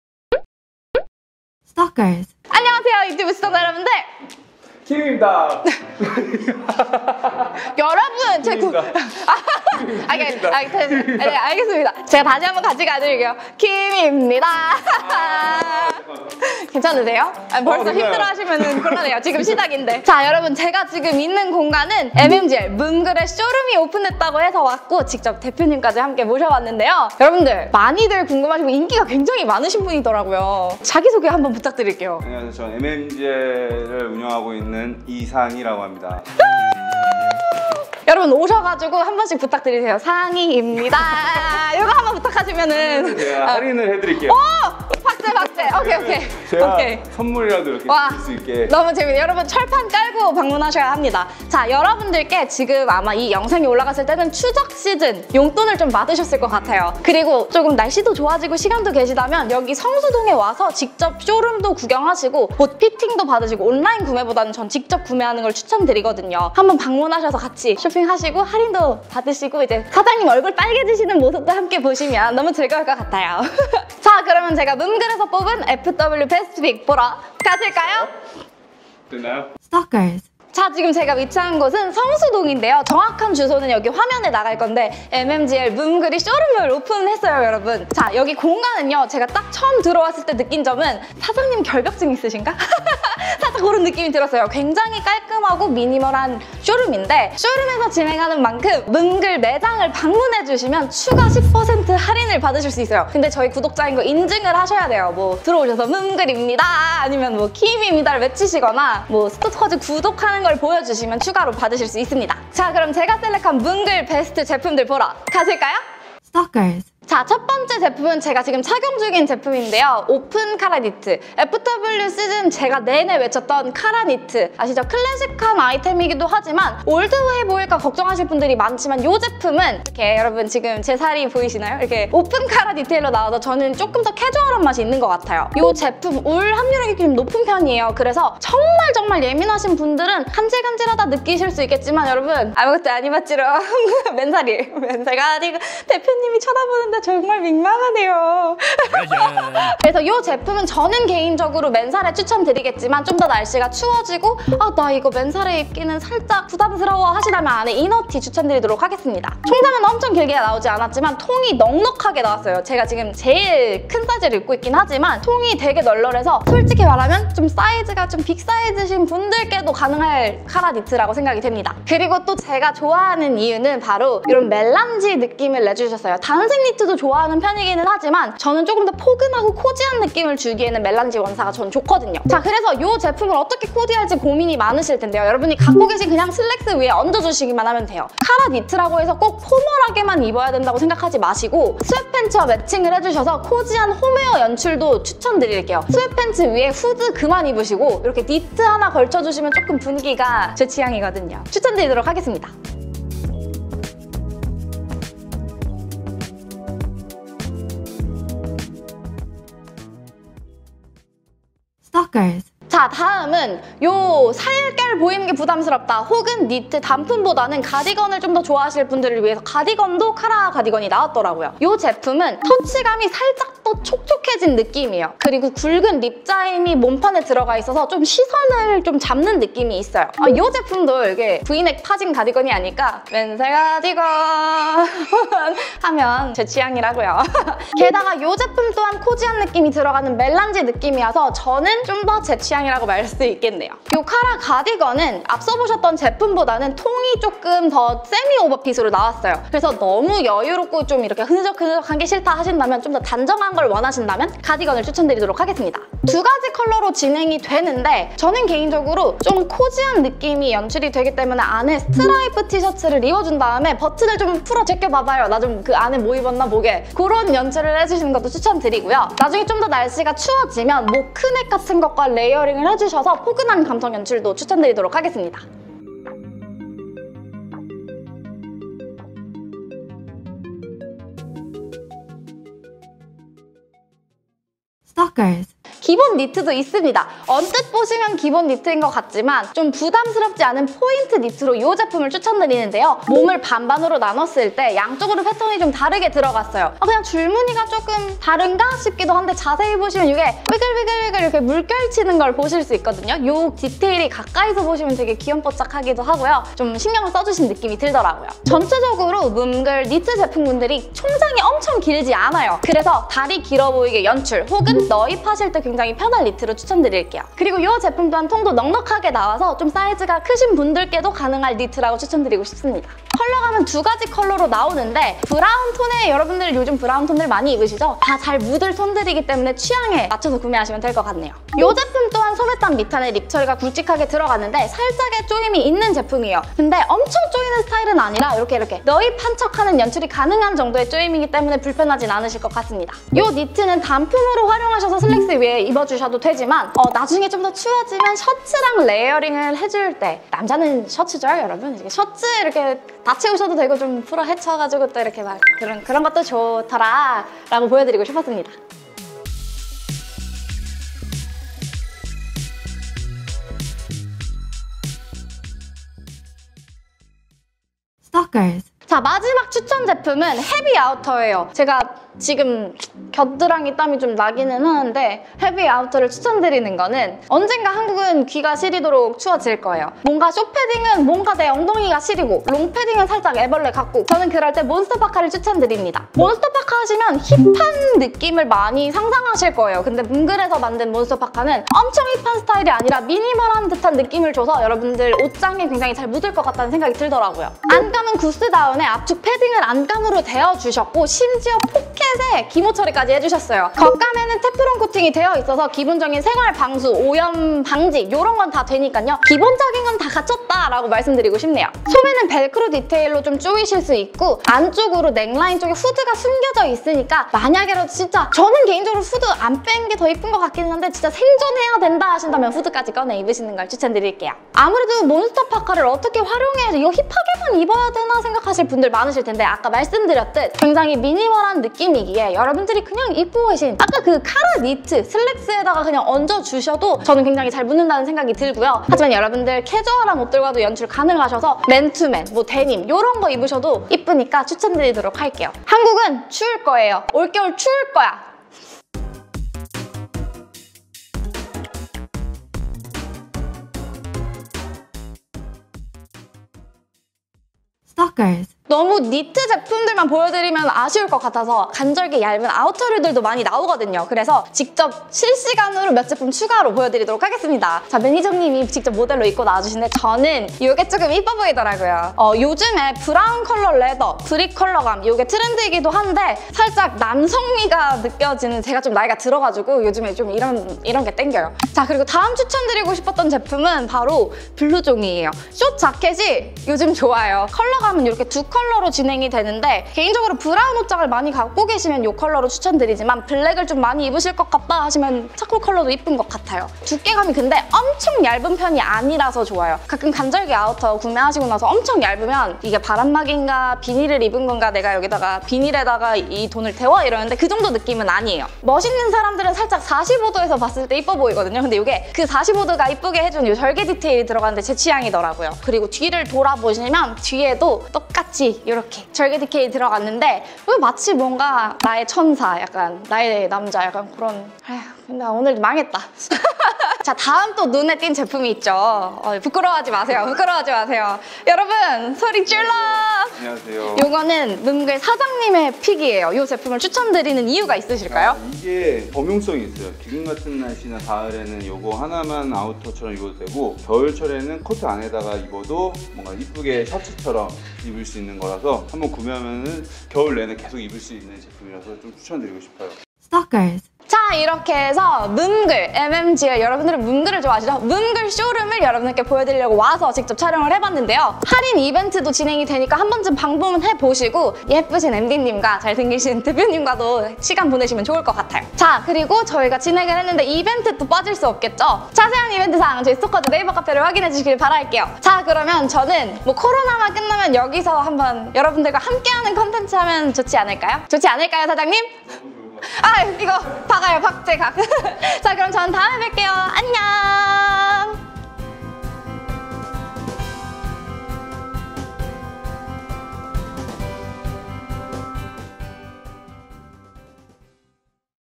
올 가을,겨울 미니멀 깔끔한 남친룩 코디하기 좋은 브랜드! (feat. MMGL)
***오늘 MMGL의 금전지원과 제품지원을 받아서 만들어졌습니다***
**영상 속 가격은 쇼룸에 방문하셔서 스토커즈 구독자 인증 후 구매하실 때의 할인가입니다!

MMGL 쇼룸 주소 : 서울특별시 성동구 성수일로1길 3, 3층 MMGL

오늘은 MMGL 쇼룸에 방문해서 FW 신상제품을 보고왔습니다!
쇼룸 방문시 스토커즈 구독자 인증하시면 추가 할인까지 해준시다고 하시니
쇼룸에서 직접 피팅도 해보시고 할인 받으셔서 현명한 소비하셨으면 좋겠습니다!

(1) [MMGL] 오버사이즈 카라 스웨터 (애쉬 브라운) -모델 4 사이즈 착용

(2) [MMGL] 오버사이즈 카라 스웨터 (페블 베이지) -모델 4 사이즈 착용

(3) [MMGL]울 카라 가디건 (다크 그레이) -모델 4 사이즈 착용

(4) [MMGL] 울 카라 가디건 (멜란지 베이지) -모델 4 사이즈 착용

(5) [MMGL]  2패턴 울 스웨터 (베이지) -모델 4 사이즈 착용

(6) [MMGL]  2패턴 울 스웨터 (올리브) -모델 4 사이즈 착용

(7) [MMGL]구스 몬스터파카 (그레이) -모델 4 사이즈 착용

(8) [MMGL]구스 몬스터파카 (블랙) -모델 4 사이즈 착용

0:00 오프닝
1:19 믐글 쇼룸 소개
1:56 오버사이즈 칼라 스웨트
3:22 오버사이즈 칼라 스웨트 - 매니정 코멘트
3:31 울 칼라 가디건
4:35 울 칼라 가디건 - 매니정 코멘트
4:44 2 패턴 울 스웨트
5:58 2 패턴 울 스웨트 - 매니정 코멘트
6:07 몬스터 파카
7:28 몬스터 파카 - 매니정 코멘트
7:37 쇼룸 - 베지터블 레더 자켓
8:01 쇼룸 - 오버사이즈 싱글 라이더
8:46 쇼룸 - 울 트러커 자켓 (사장님 픽)
9:12 엔딩